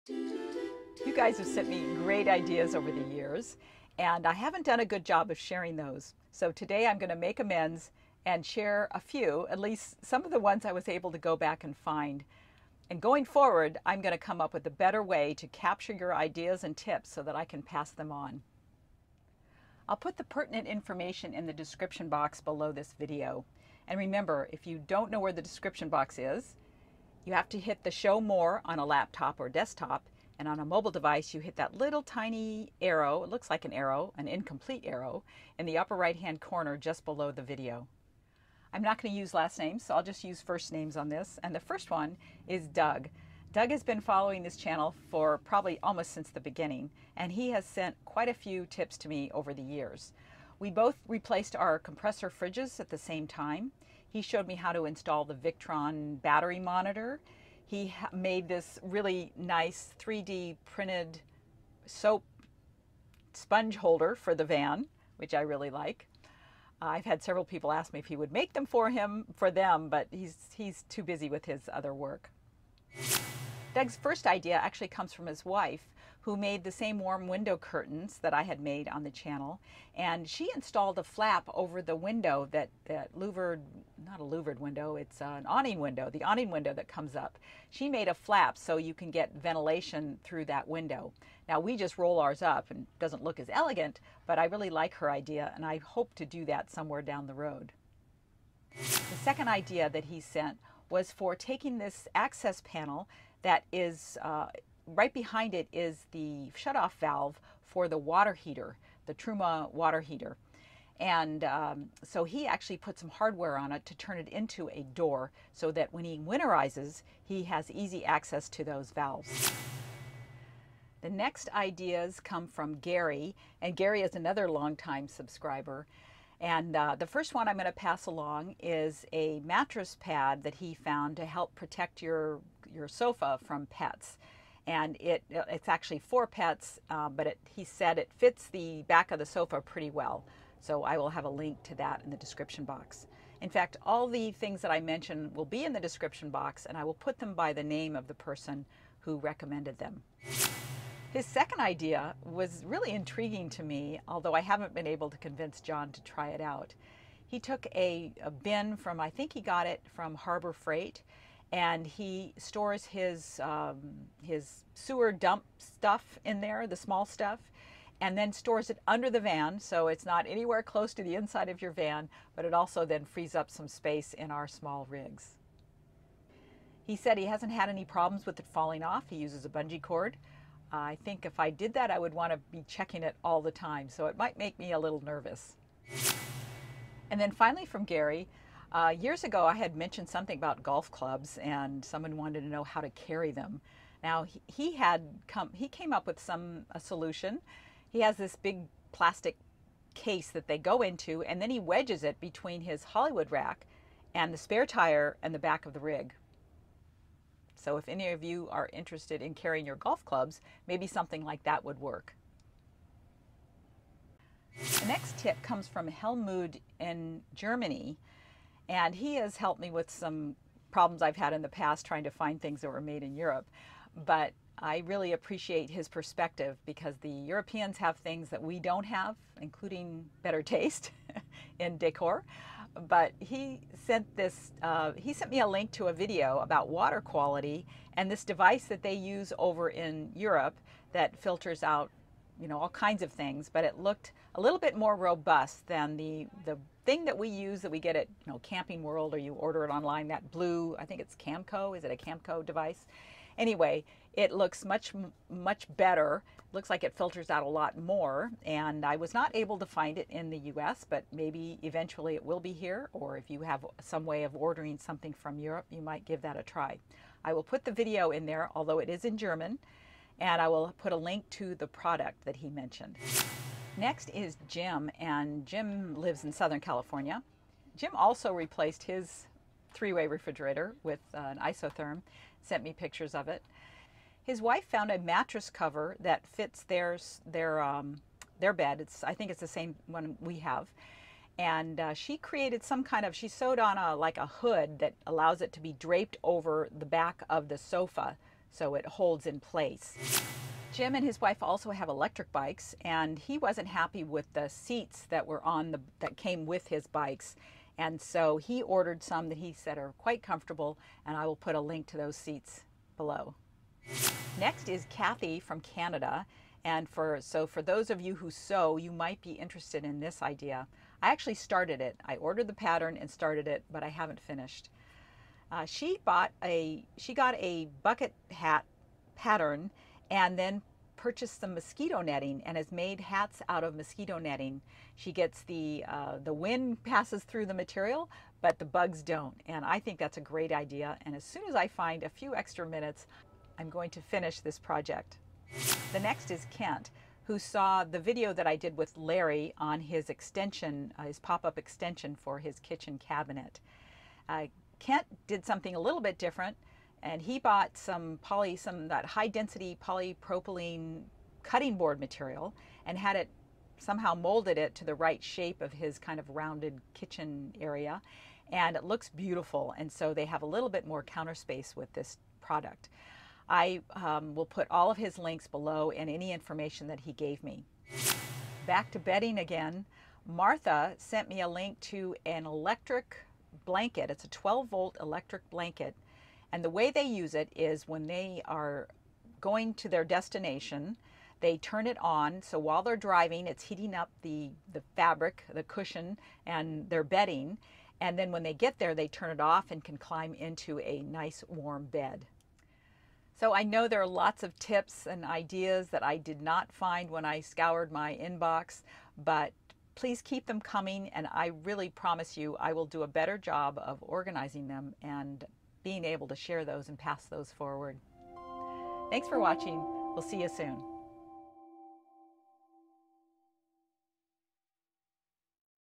11 Hacks/mods/tips/inspiration for your campervan: Shared by Viewers
I receive lots of great ideas from my viewers to make travel in our Pleasureway campervan more enjoyable.  Today I am passing them forward.

Under carriage storage: " I looked up under the van and found the forward support strut near the front driver's door.  I found the large size plastic Husky bin at Home Depot and it allows a snug fit on top of the strut.  I bungeed it to the strut to secure it a firmly.  The bin cost about $6. Could be used for other items as well. "
golf club storage

Jim:
Ikea mattress cover
Bikeroo oversized comfort seat

Kent
"We used a router with a 3/8 inch cove bit to machine the mating edge to the existing Corian bullnose. The support legs were bent from 3/8 inch aluminum tubing including the nylon leg feet to prevent marring the existing cabinet.  When not in use, it will be stored on edge in our closet."
cutting board material
table support
Martha